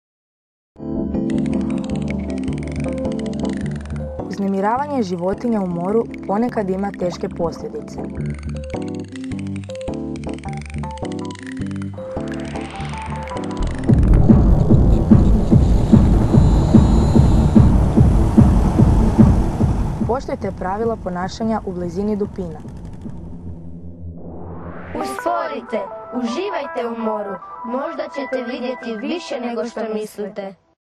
Disturbance and harasment endangers marine organisms
A small add reminding you that sea is home to diferent organisms. In order to feed, breed, rest or raise their offsprings they need their privacy. Therefore, try to minmize your impact by reducing your speed and by following the code of condust in the presence of dolphins.